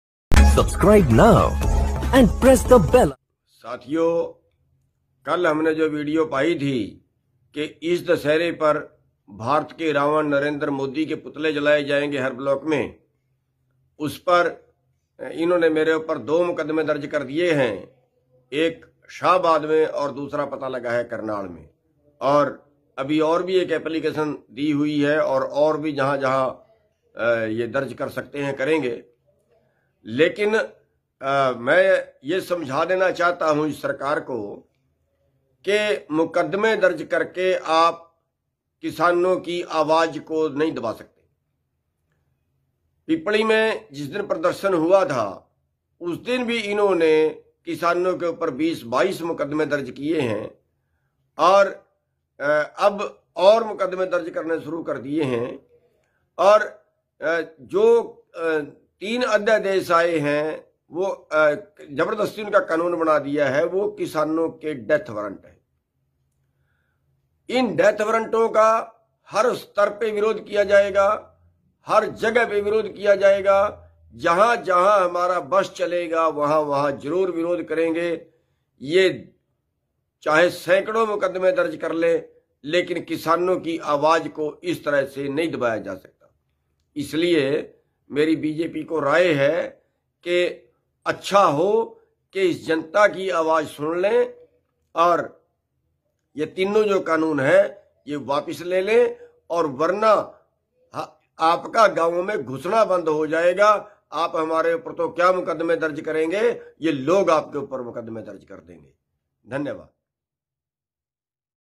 बड़ा बयान: जितने मर्जी मुकदमे दर्ज कर लो लेकिन दशहरे वाले दिन मोदी के पुतले हर हाल में जलाए जाएंगे
जितने मर्जी मुकदमे दर्ज कर लो किसानों की आवाज नहीं दबेगी आंदोलन भी ऐसे ही चलेगा और दशहरे वाले दिन मोदी के पुतले भी जलाए जाएंगे